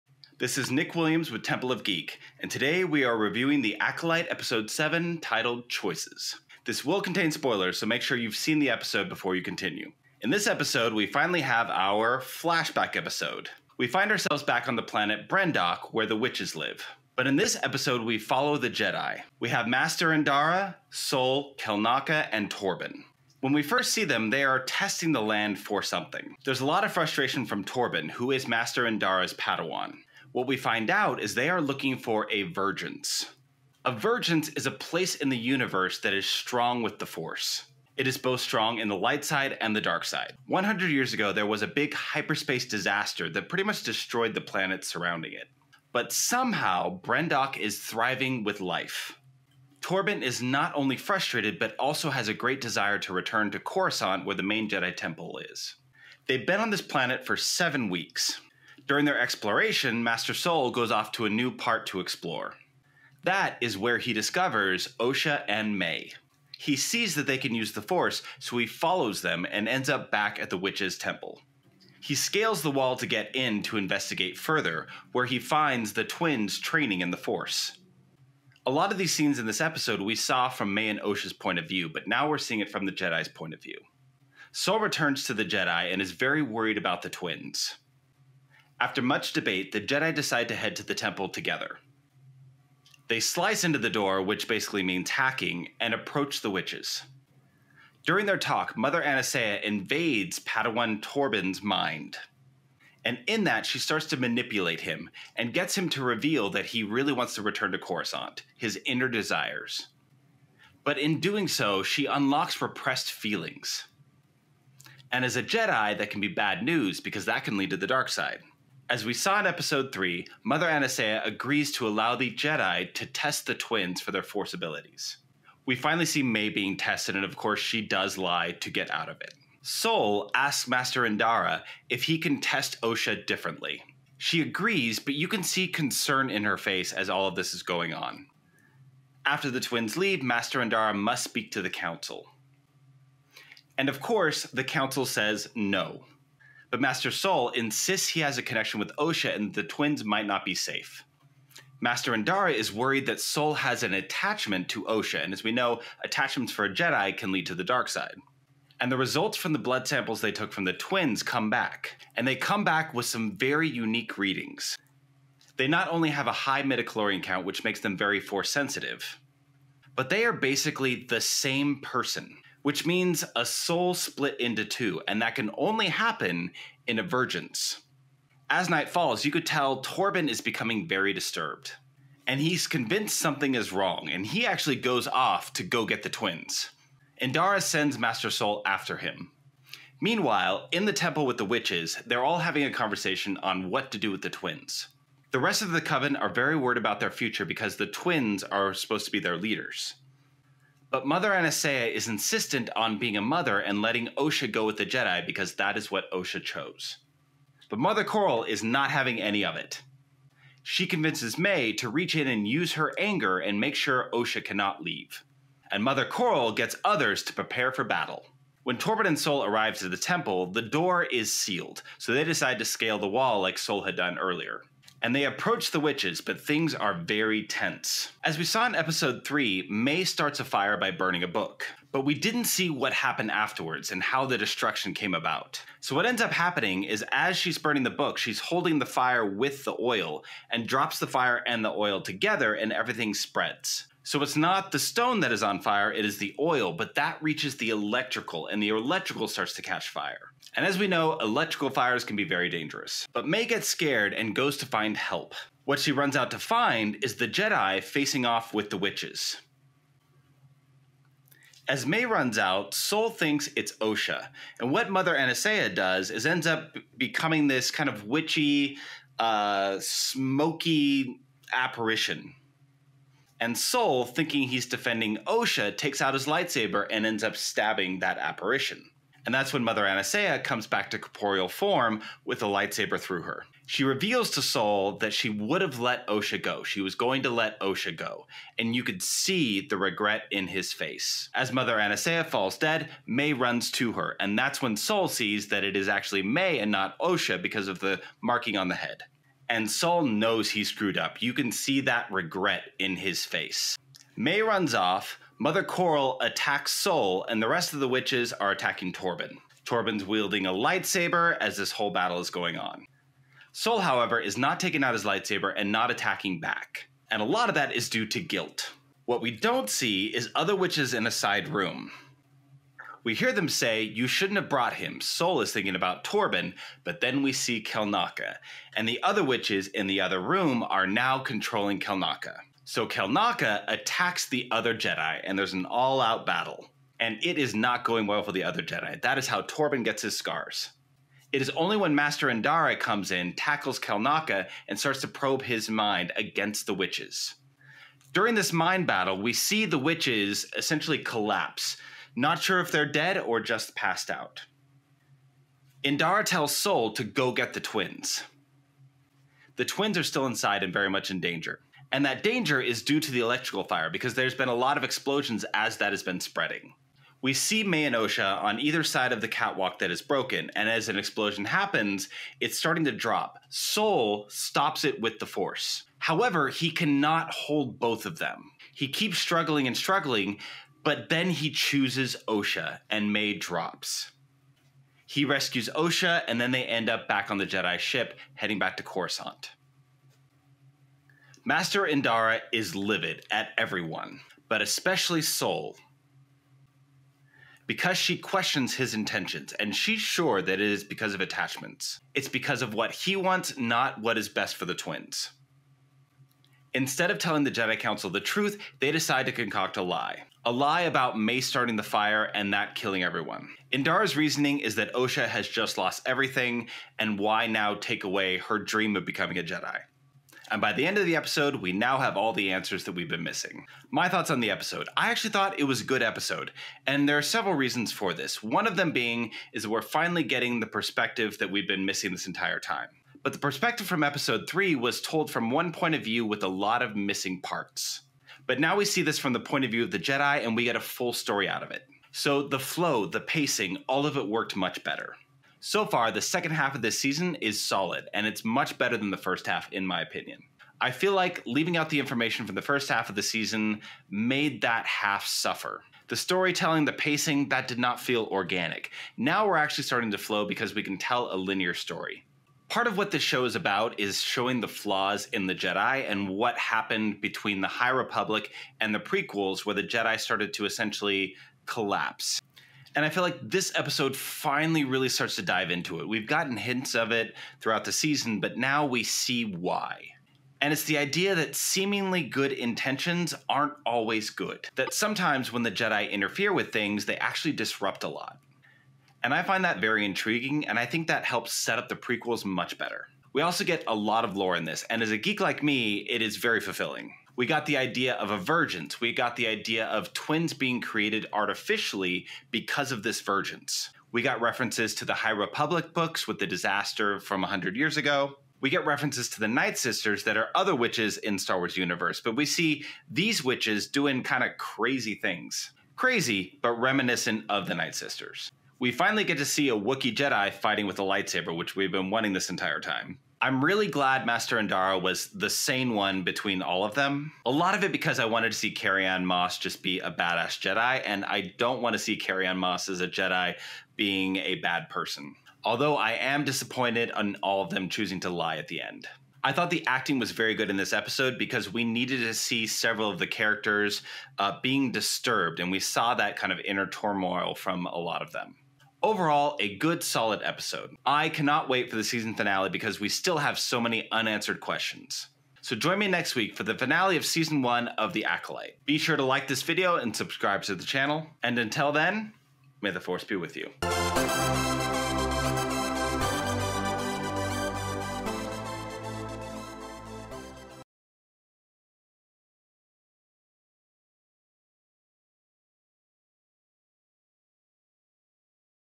Recap & Review: Star Wars: The Acolyte, Season 1, Episode 7, "Choice"
In this episode of Temple of Geek, Nick Williams reviews episode seven of The Acolyte, titled "Choices." This episode is a flashback to the planet Brendock, where the witches live, and follows the Jedi as they search for a vergence, a place strong in both the light and dark sides of the Force. The Jedi discover the twins, Osha and Mae, who can use the Force, and become concerned for their safety. The episode explores the flaws in the Jedi and the events leading up to their collapse, as well as the complex relationships between the Jedi and the witches. The reviewer praises the episode for its pacing, storytelling, and the answers it provides to previously unanswered questions. He also highlights the episode's exploration of the Jedi's flaws and the dangers of attachment. Overall, the reviewer finds the episode to be a solid addition to the series and eagerly anticipates the season finale.

For more content that pertains to the world of geek visit our